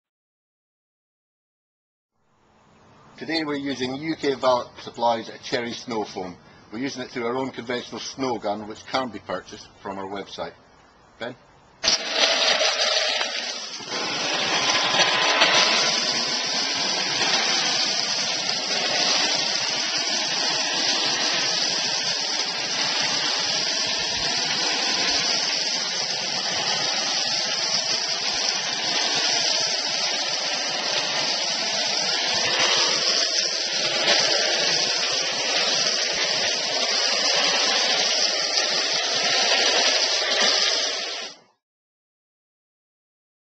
UK Valet Cherry Snow Foam
UK Valet Cherry Snow Foam is a special vehicle cleaning preparation for use with foaming equipment.

This foam may be left in place for a few minutes in order to derive a prolonged cleaning effect and is easily rinsed away with water. This product can also be used in high pressure machines.